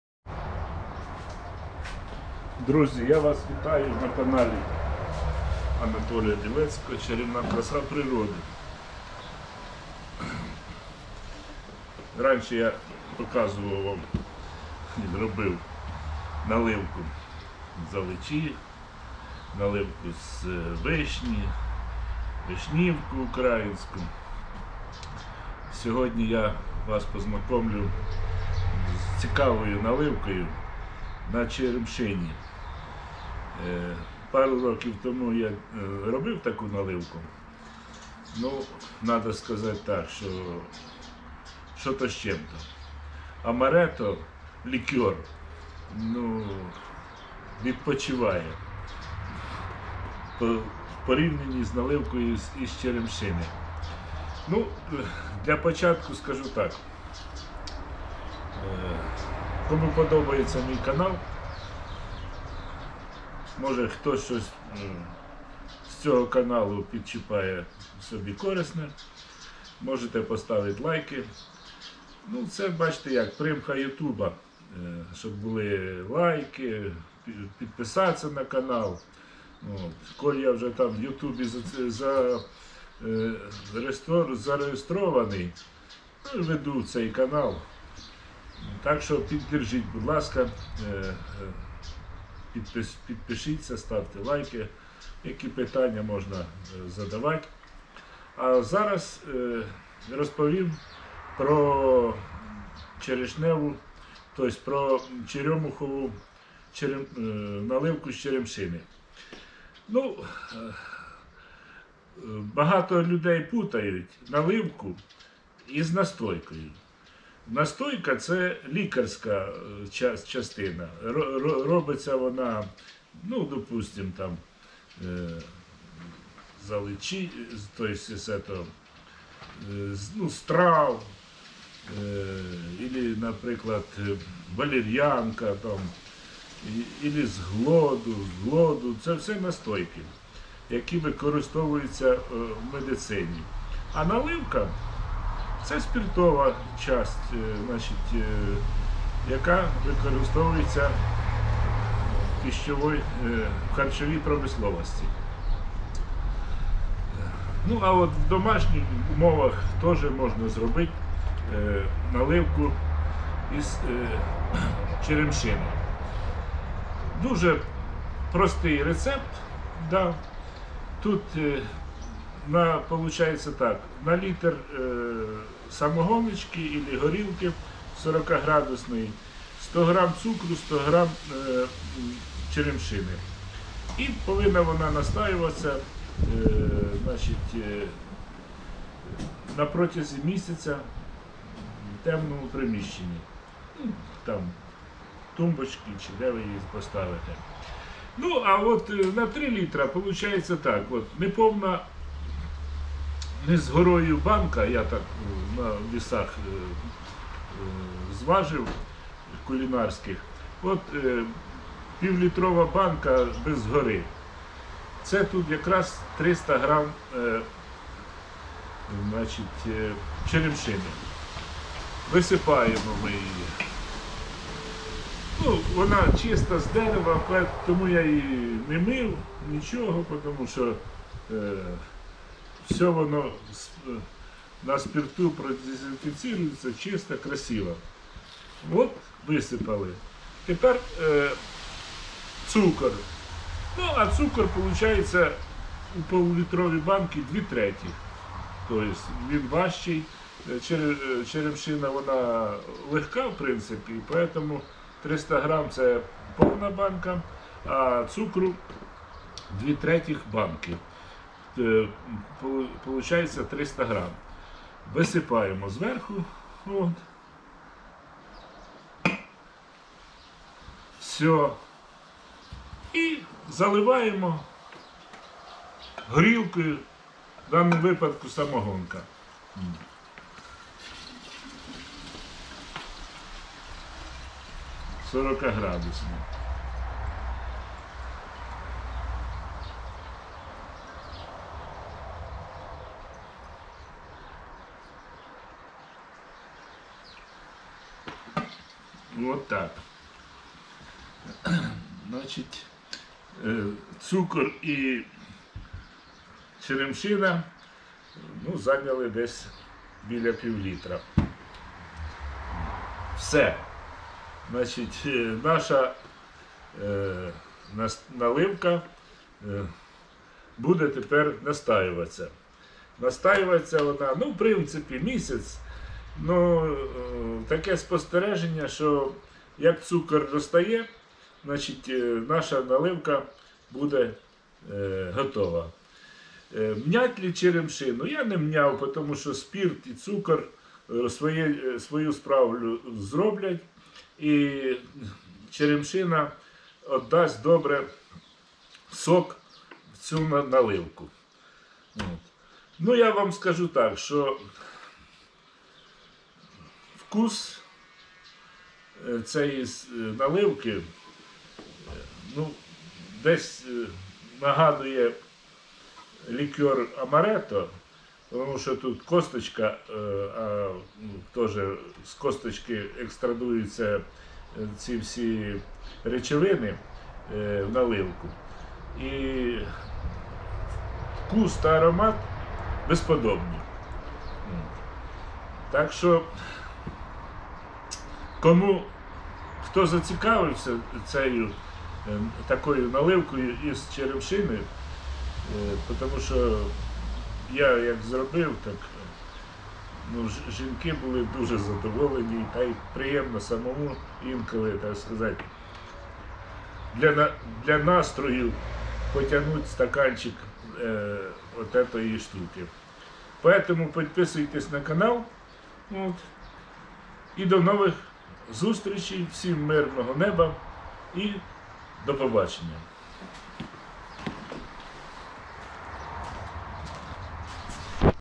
Наливка із черемшини- смакує як лікер амарето.
Проста в приготувані та  смачна в споживані. наливка із плодів черемшини. Стоїть якраз пора дозрівання   черемшини. Не пропустіть  остані осінні плоди та порадуйте себе  в зимовий період за бокалом цього чудового напою спогадами про тепле літечко.